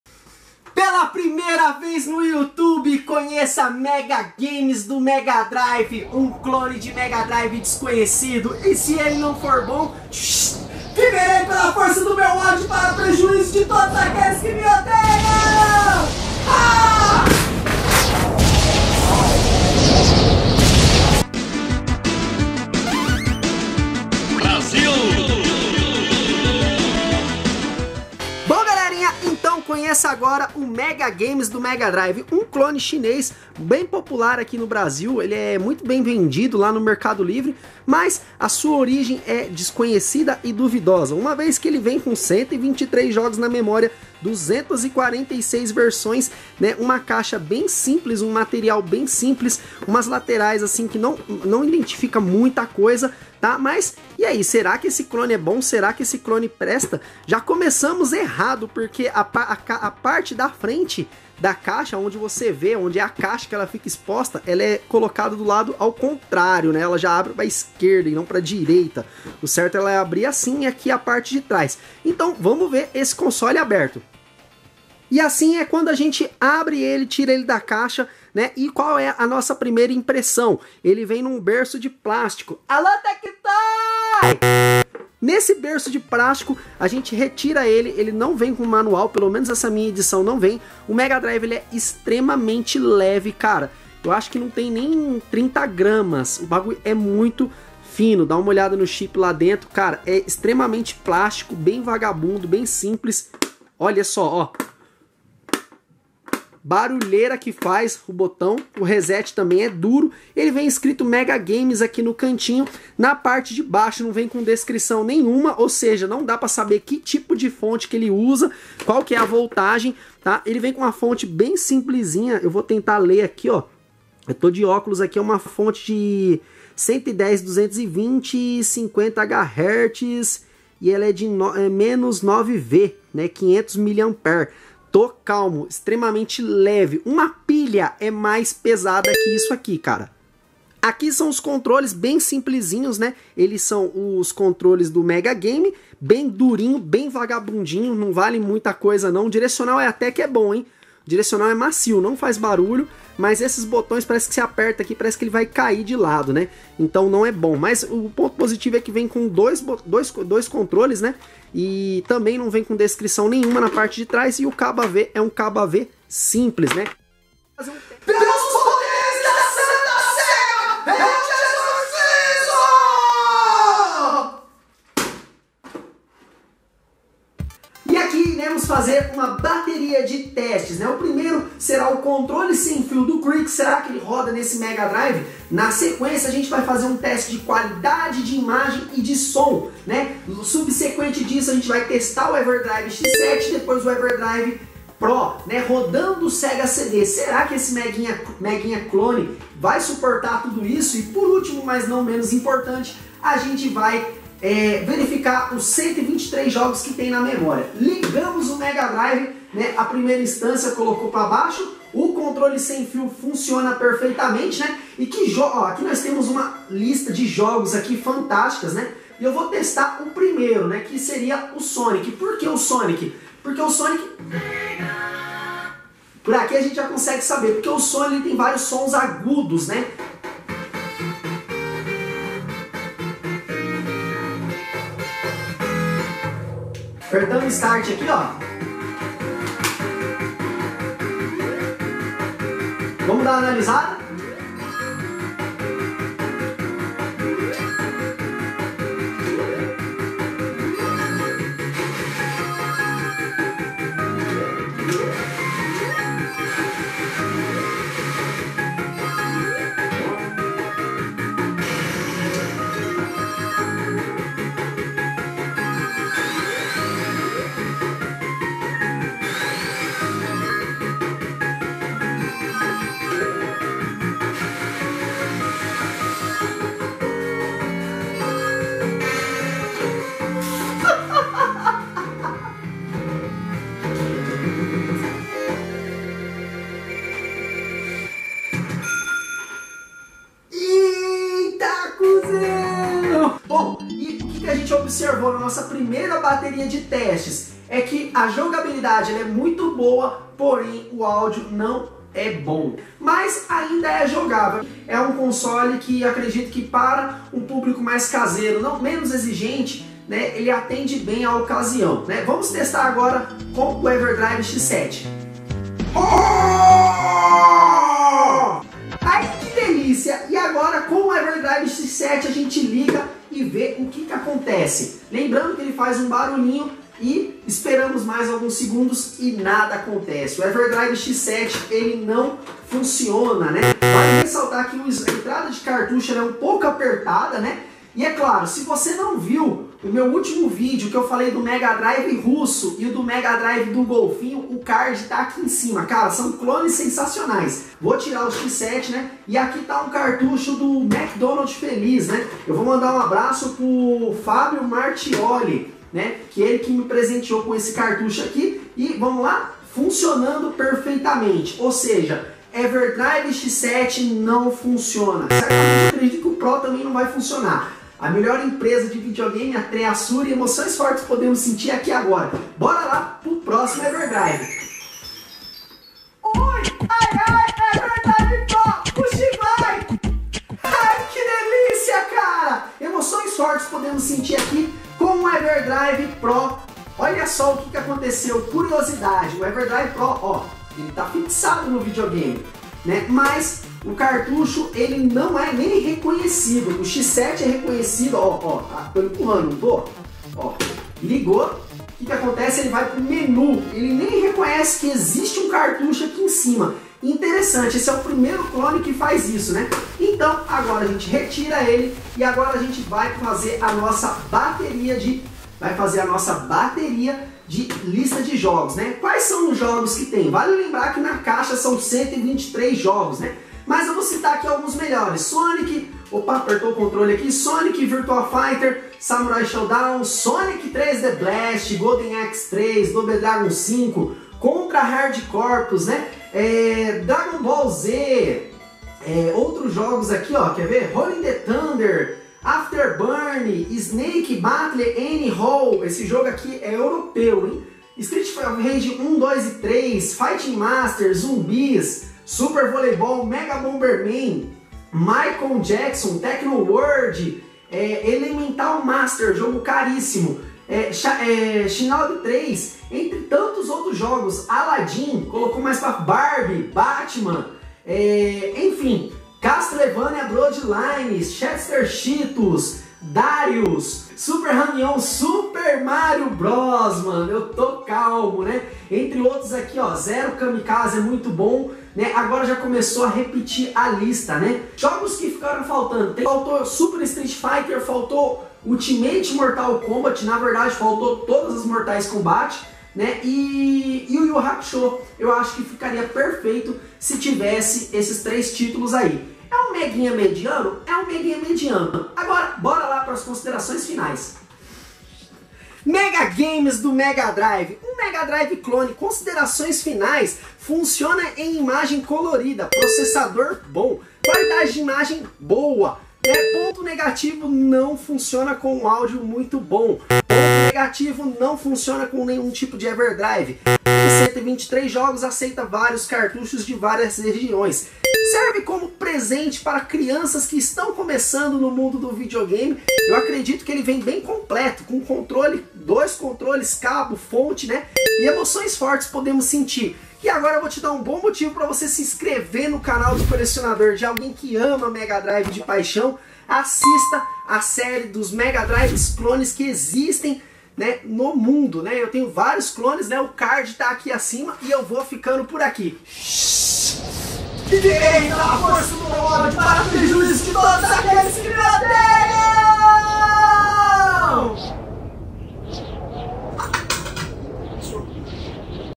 MEGA GAME do Mega Drive CLONE ! UCOLECIONADOR !
MEGA GAME do Mega Drive CLONE !
MEGA GAMES É UM CLONE FABRICADO NA CHINA DISTRIBUIDO O BRASIL, , O CONSOLE TEM UM VISUAL INTERESSANTE MUITOS PONTOS POSITIVOS PARA UM MEGA DRIVE CLONE , MEGA GAME TEM 123 JOGOS NA MEMORIA E 246 VERSOES , É UM MEGA DRIVE INTERESSANTE PARA SE JOGAR OU DAR DE PRESENTE , ALEM DO MEGA DRIVE MEGA GAME RECOMENTA VOCE ASSISTIR O STAR DRIVE 2 DO MEGA DRIVE UM SUPER CLONE  !

YouTube Canal: UColecionador !